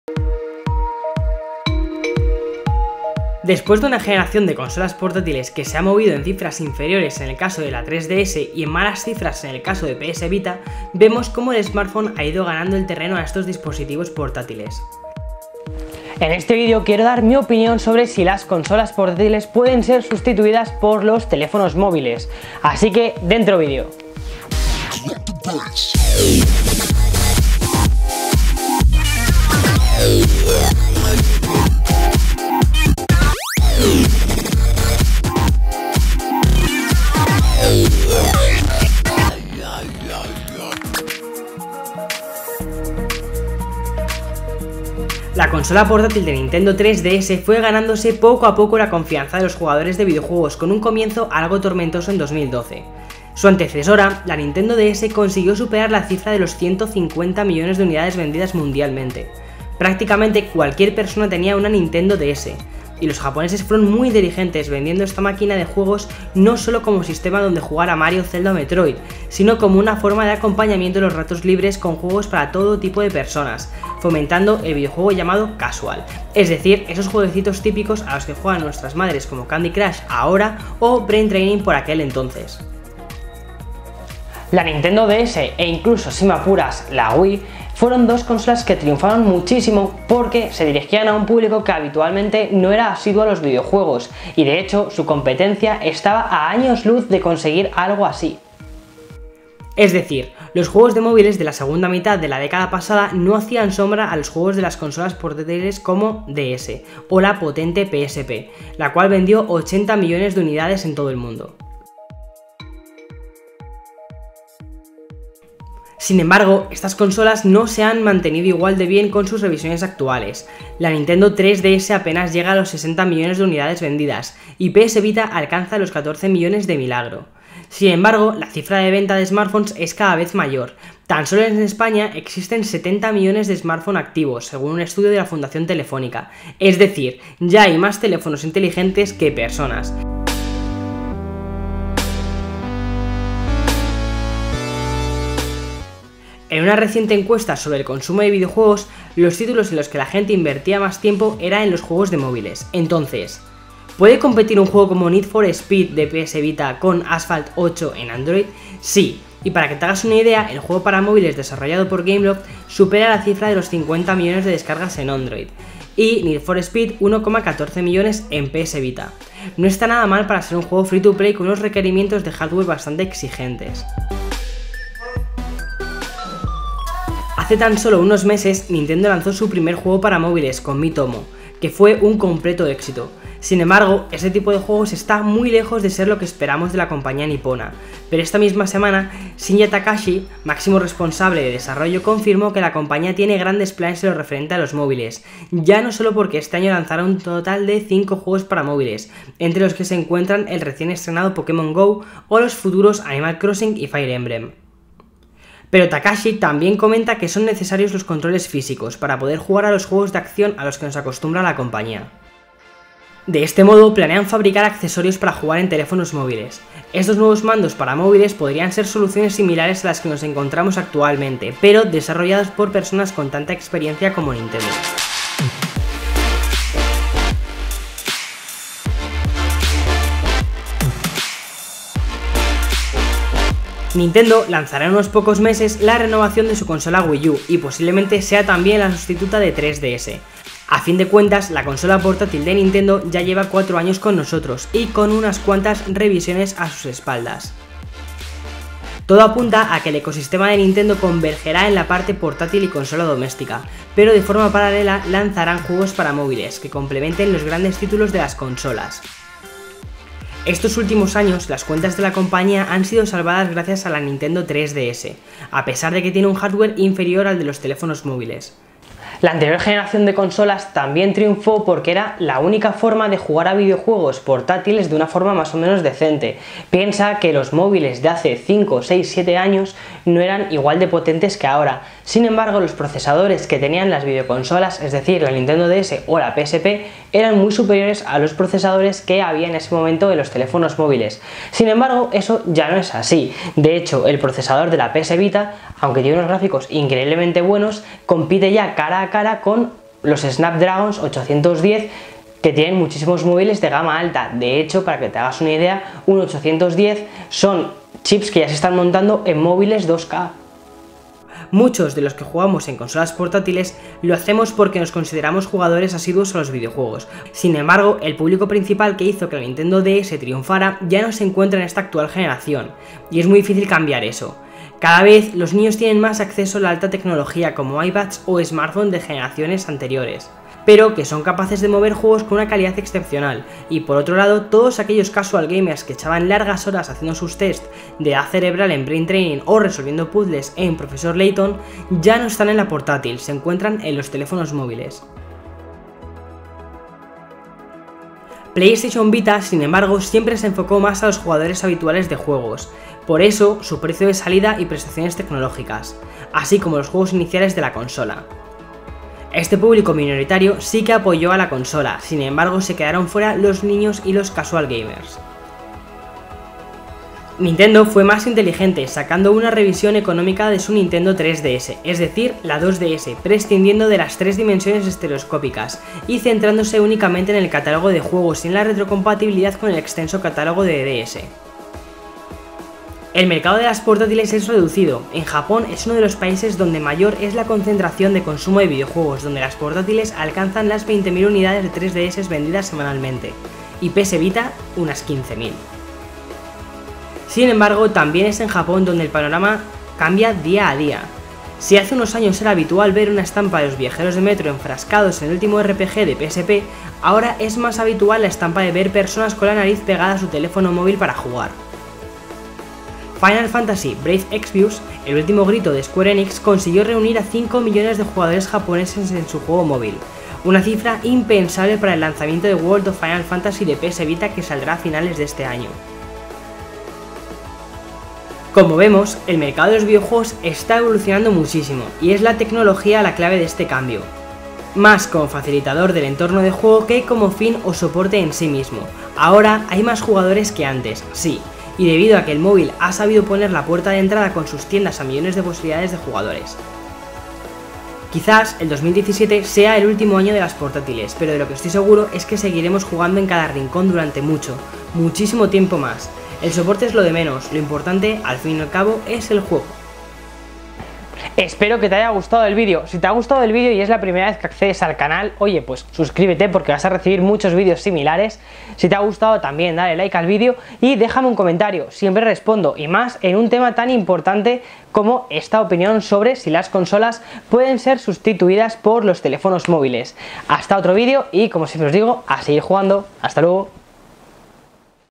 ¿Pueden los móviles sustituir a las consolas portátiles PS Vita o 3DS? | Opinión
Los nuevos iPhone, Samsung Galaxy S7 y otros móviles Android son tan potentes que pueden plantar cara a PS Vita o Nintendo 3DS. Te cuento el futuro de las videoconsolas portátiles, y los juegos más importantes de móviles que tienen calidad de ser competidores de consolas.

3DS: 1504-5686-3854
Wii U: VictorAbarca
PSN: VictorAbarca